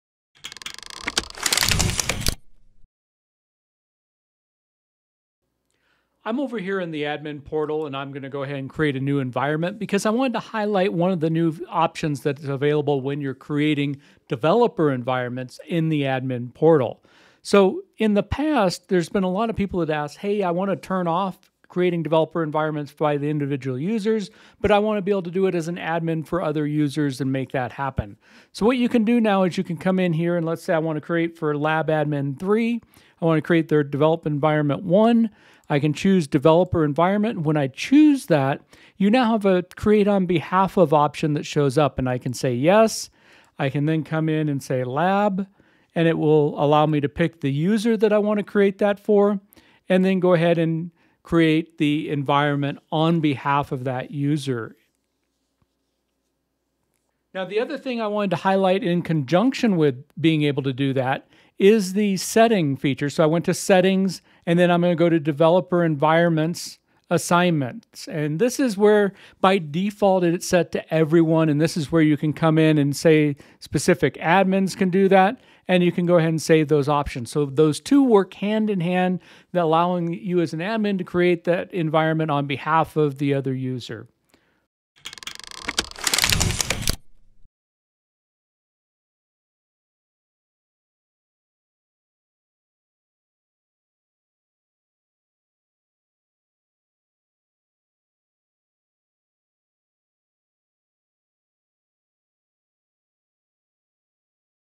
Creating Power Platform Developer Environments On Behalf Of | 365.Training
In this video, David Yack walks us through how you can now create developer environments for others. You can also change organizational settings to lock down who can create developer environments. You can also use the on behalf of feature from the Power Platform CLI.